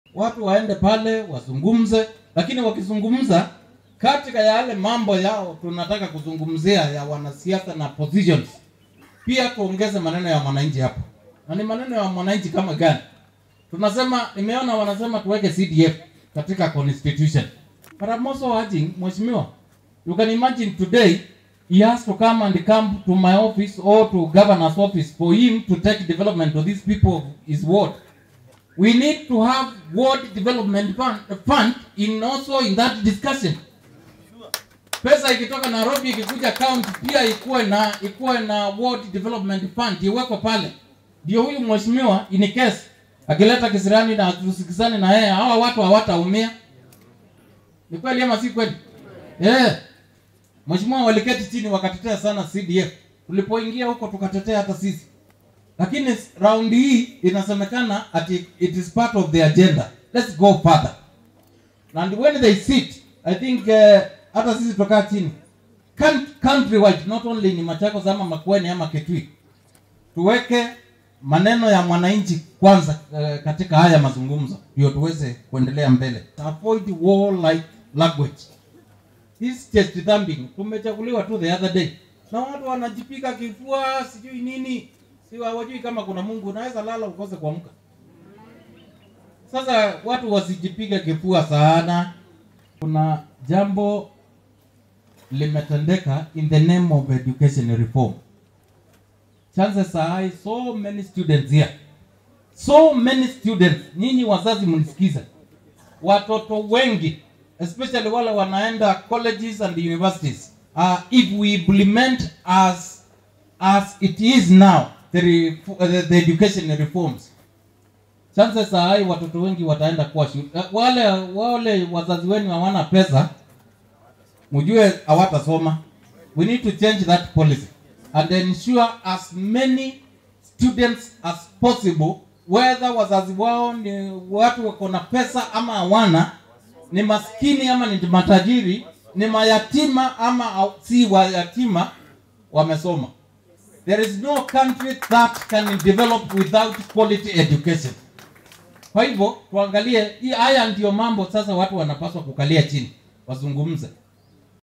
Mwangangi: The Bipartisan talks should advocate for a law to introduce Ward Development Fund!
Machakos Deputy Governor Francis Mwangangi now wants the Committee handling talks between the azimio la umoja and Kenya kwanza to spherhead mwananchi based talks other than dwelling on issues touching on political heavyweights. Key among the issues Mwangangi wants addressed is the introduction of a Ward Deputy Fund.